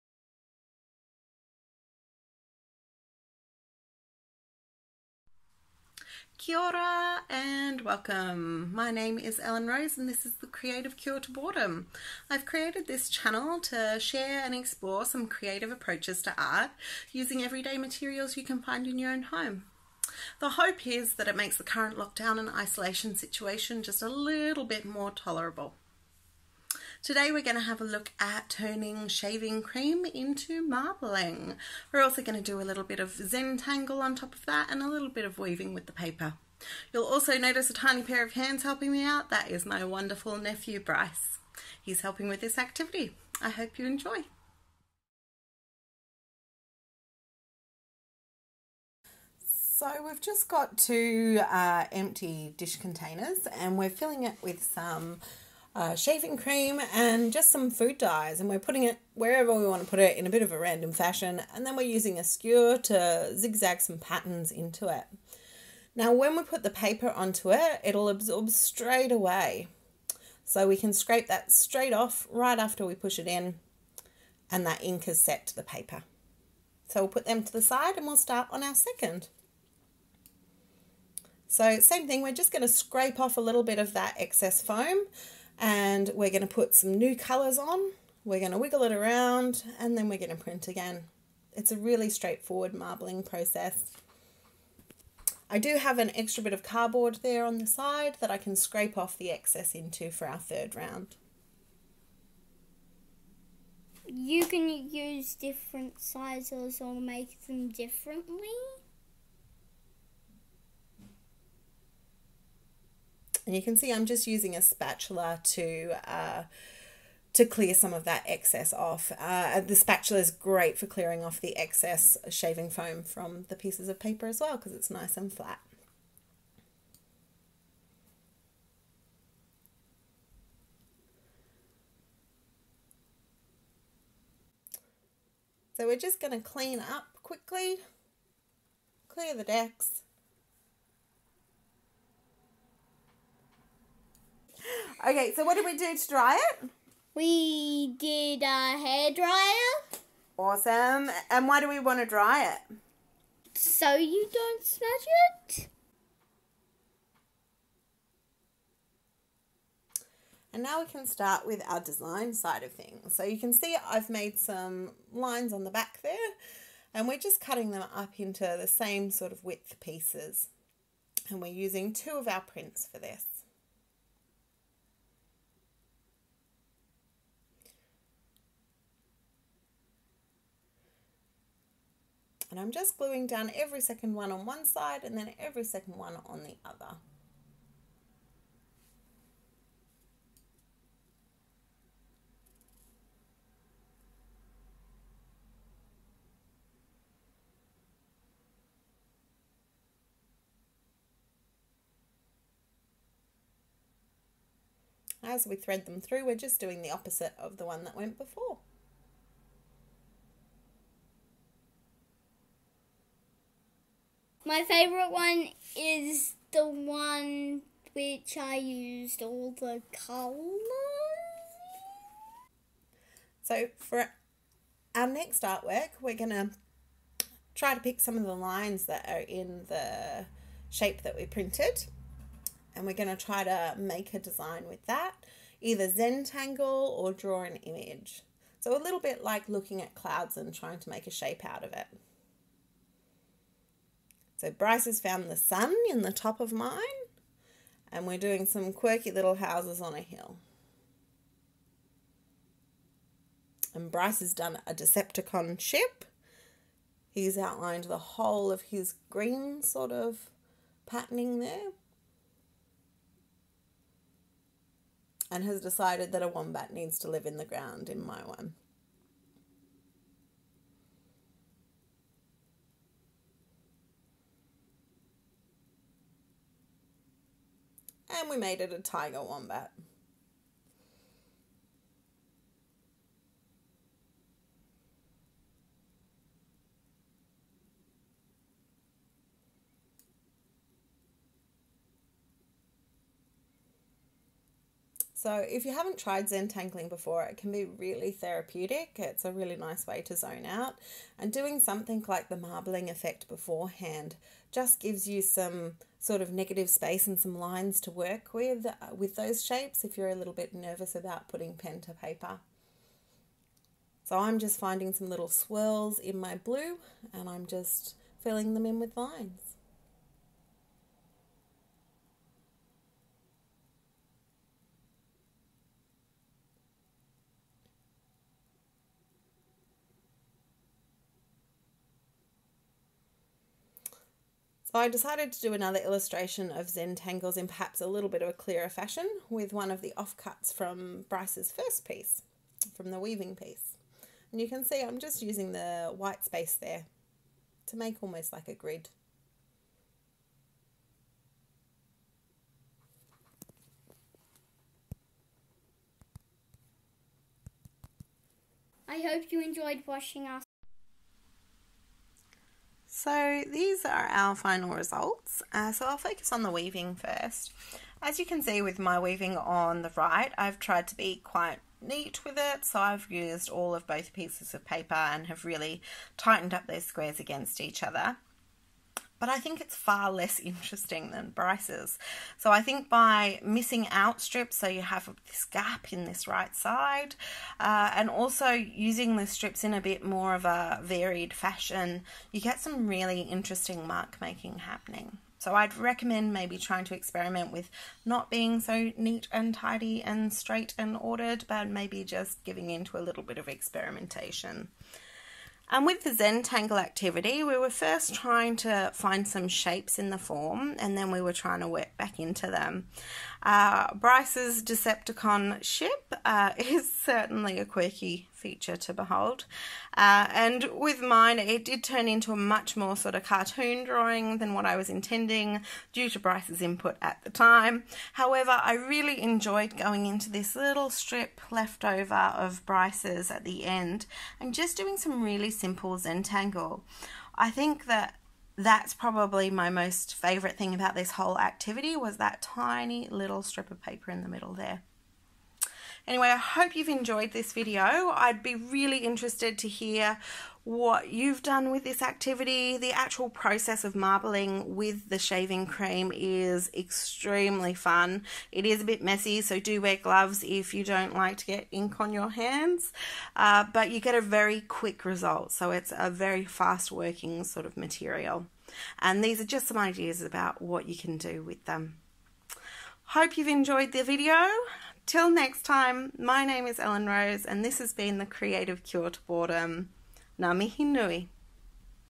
Creative Cure to Boredom: shaving cream to marbling
This video explores the use of shaving cream and food dye to create a simple marbling technique on paper.